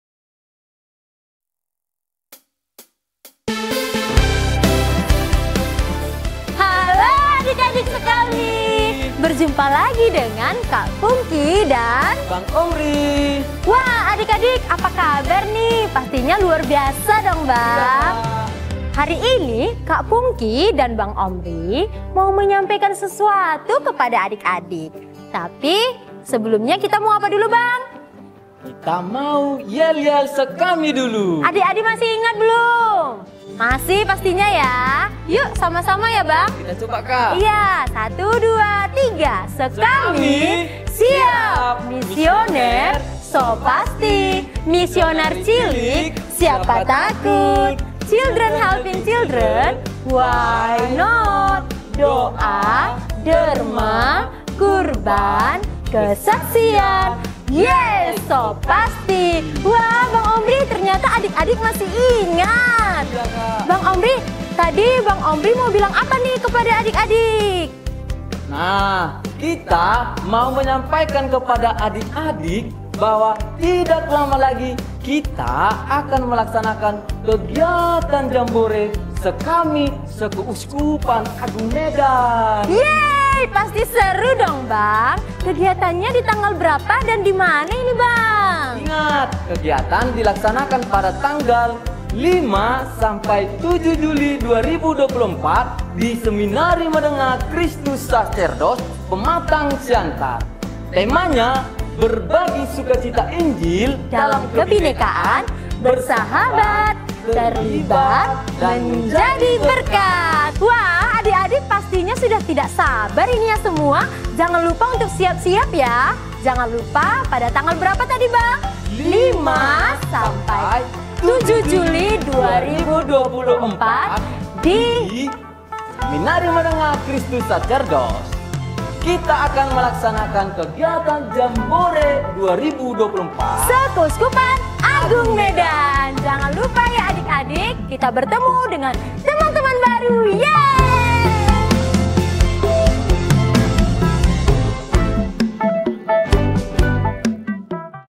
COMING SOON JAMBORE SEKAMI KAM 2024
JAMBORE SEKAMI KAM 2024 akan diselenggarakan di Seminari Menengah Christus Sacerdos, Pematang Siantar (5-7 Juli 2024).
Peserta : Serikat Kepausan Anak Misioner (Sekami) se-KAM